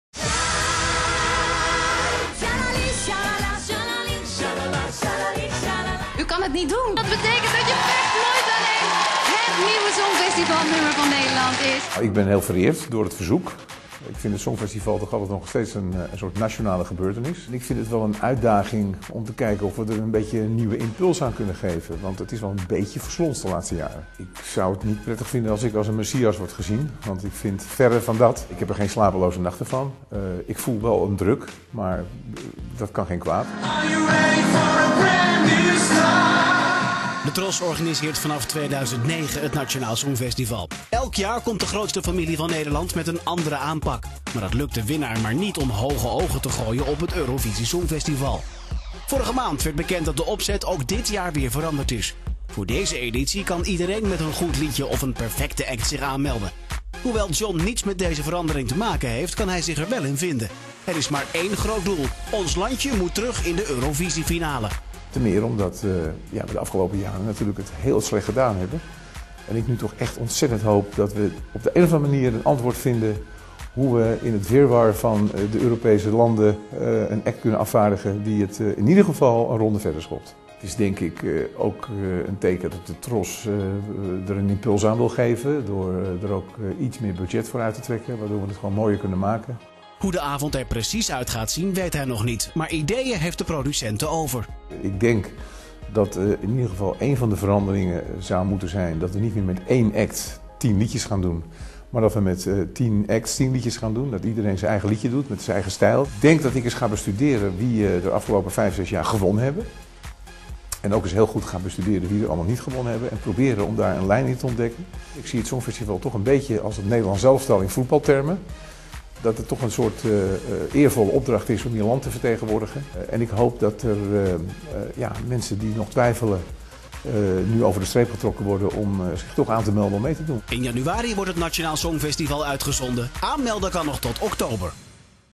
John de Mol producer Nationaal Songfestival 2012 (subtitles)
Subtitles available

John de Mol will be the producer of Nationaal Songfestival 2012, TROS announced. "John de Mol knows as nobody else how you can make a show with an international style," leader of TROS Peter Kuipers said. John de Mol will do several things including the ideas for the backdrop and the act.

John de Mol developed and produced television programs that have become popular around the world like Big Brother, Love Letters, 1 vs. 100, Deal or no deal and of course recently The Voice (of Holland).